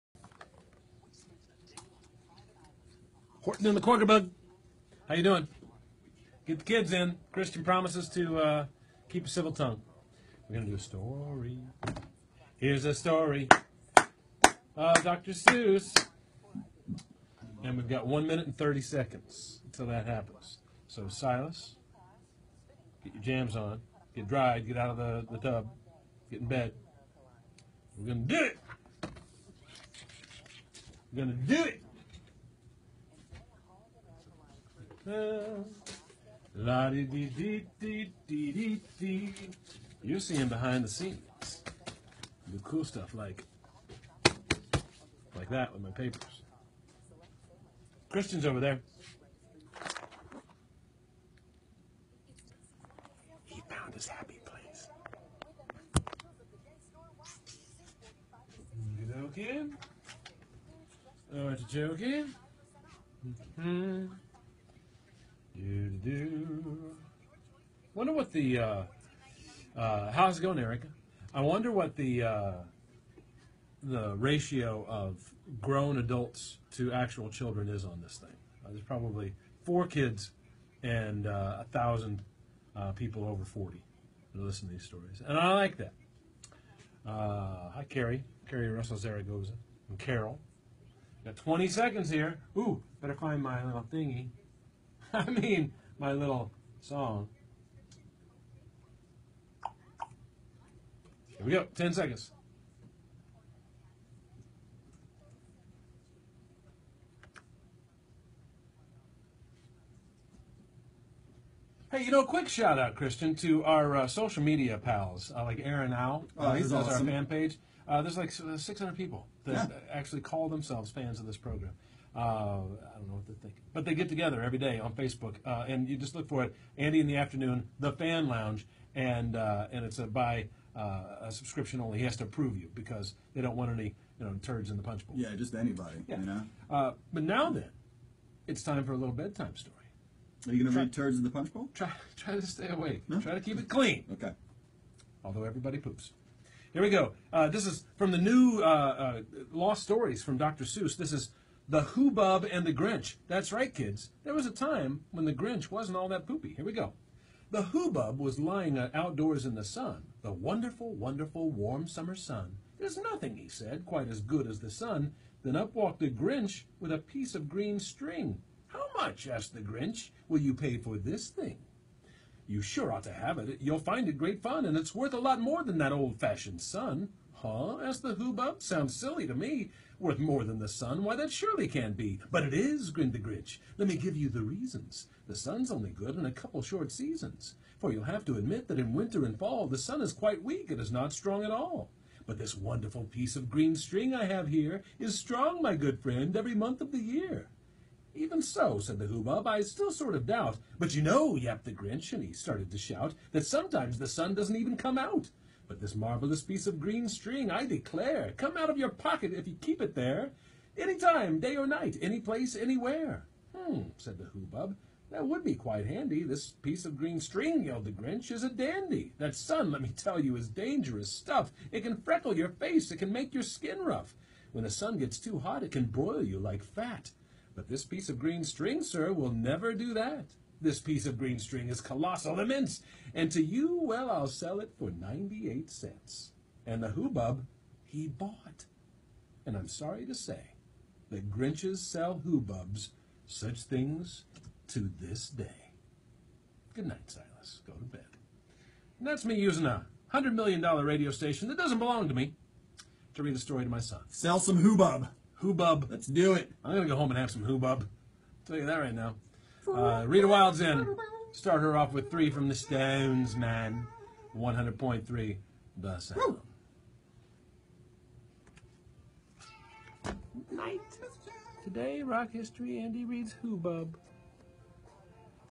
Hoobub & The Grinch - February 18, 2016 AITA Archives
Andy reads Hoobub & The Grinch from Horton and the Kwuggerbug and other lost stories by Dr. Seuss for Silas!